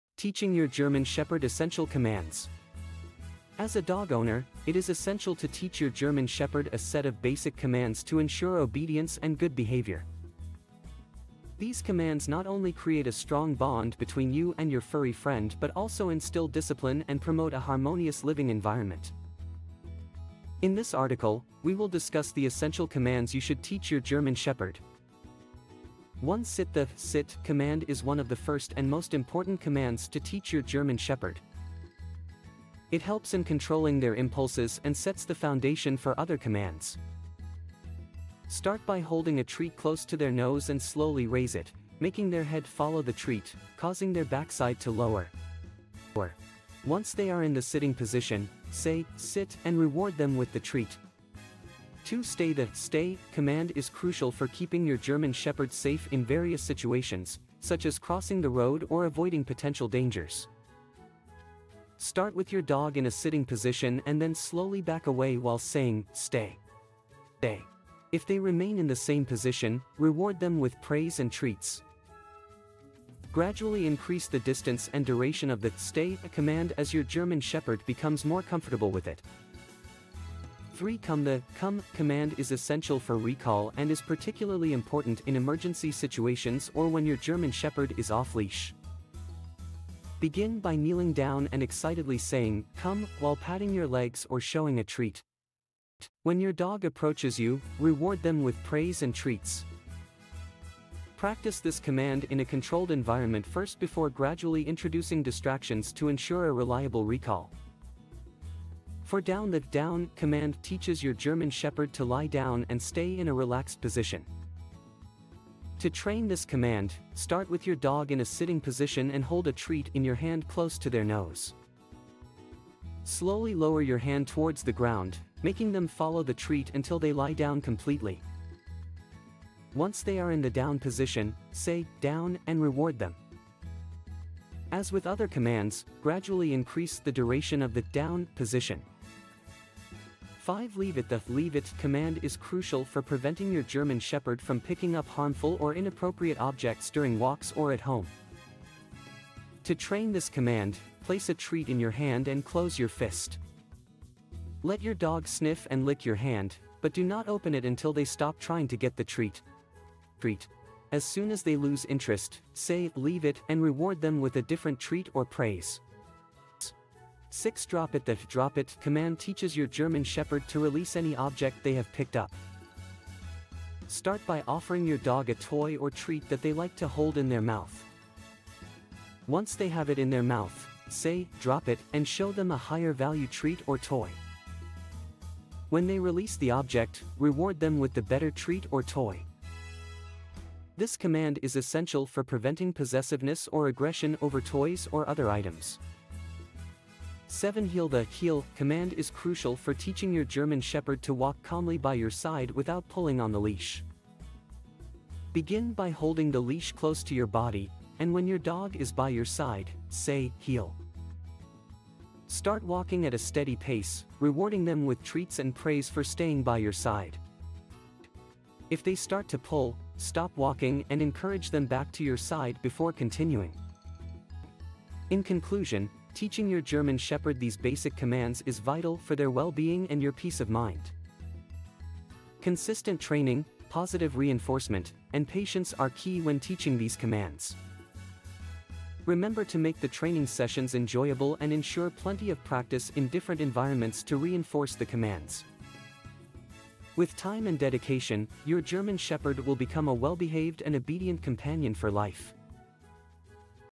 Teaching Your German Shepherd Essential Commands
As a dog owner, it is essential to teach your German Shepherd a set of basic commands to ensure obedience and good behavior  These commands not only create a strong bond between you and your furry friend but also instill discipline and promote a harmonious living environment  In this article, we will discuss the essential commands you should teach your German Shepherd

1 Sit
The "sit" command is one of the first and most important commands to teach your German Shepherd  It helps in controlling their impulses and sets the foundation for other commands
00:00 Teaching Your German Shepherd Essential Commands